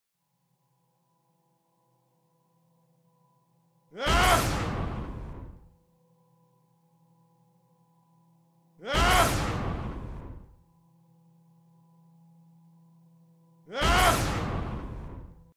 Superman Laser Test
A small animation of Ashwin Inamdar's Superman Rig with particle effects by Lee Alailima-Rose.